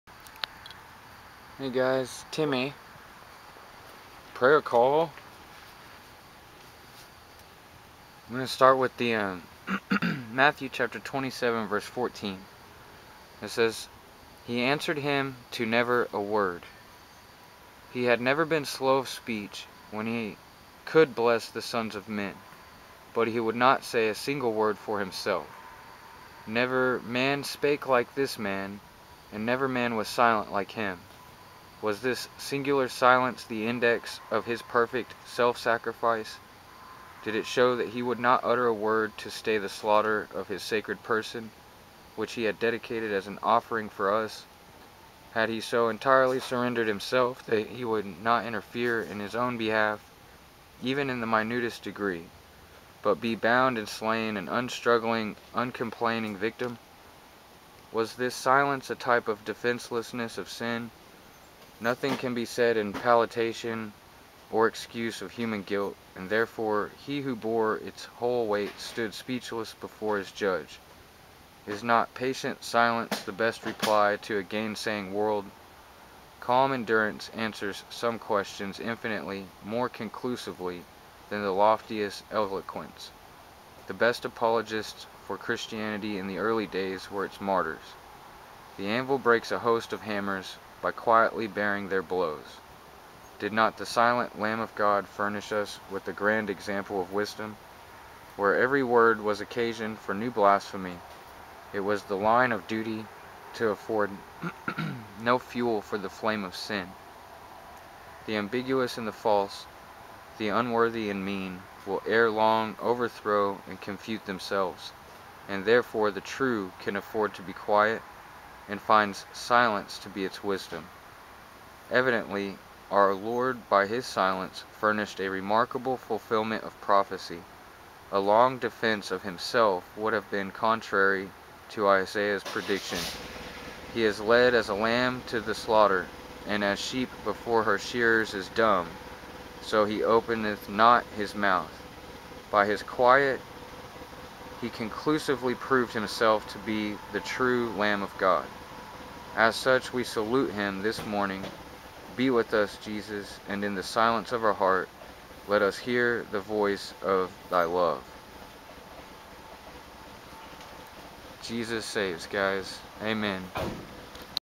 P.C.~Get SAVED Because Jesus SAVES!~4/2/16
The way to get SAVED! Mark 1:15 and saying, The time is fulfilled, and the kingdom of God is at hand: repent ye, and believe the gospel.
Mark 6:12 And they went out, and preached that men should repent.